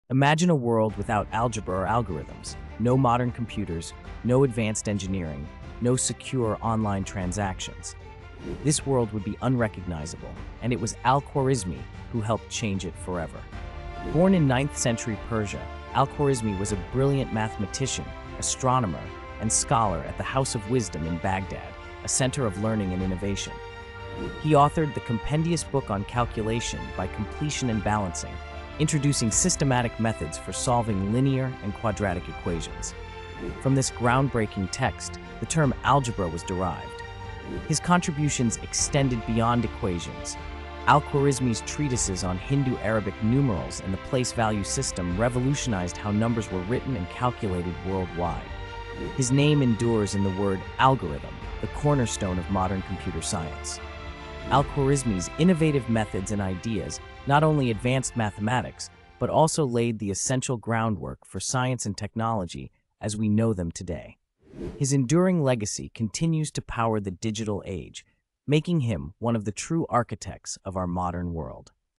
Al-Khwarizmi: The Genius Behind Algebra
Meet Al-Khwarizmi, the brilliant mind who revolutionized mathematics and shaped the modern world. Discover his legacy!